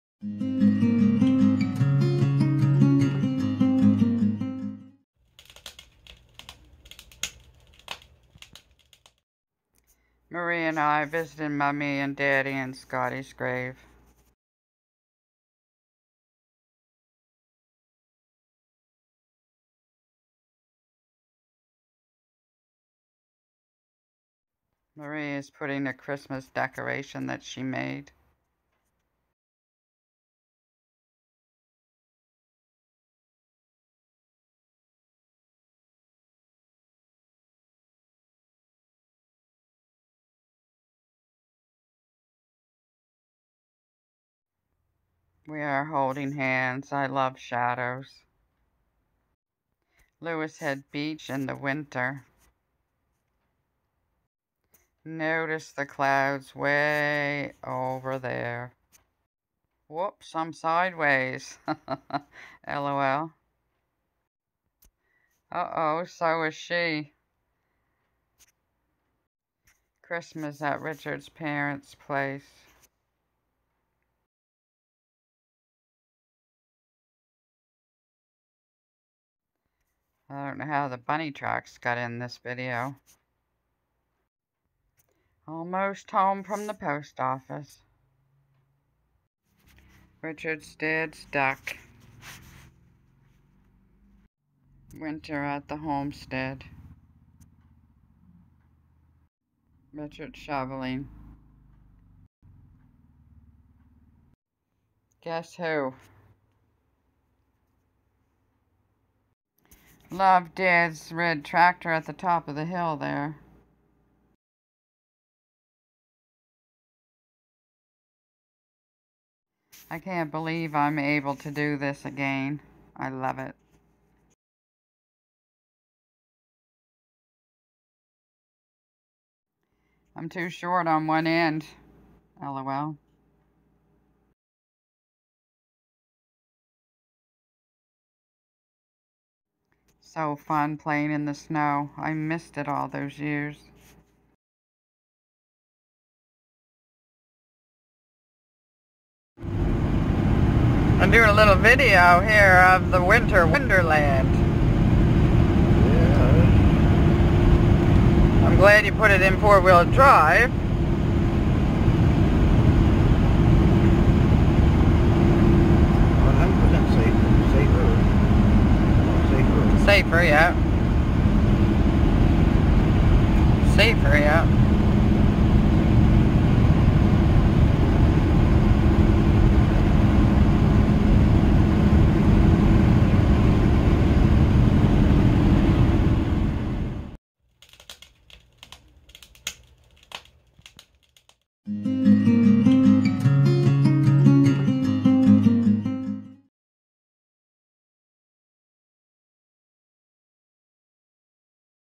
Dec2019 Marie & I visit mommy❤,daddy💙 &Scotty💙. Louis Head beach. Winter-Christmas @ the homestead
Just an old senior sharing a snowy 🎄December🎄 with family at the homestead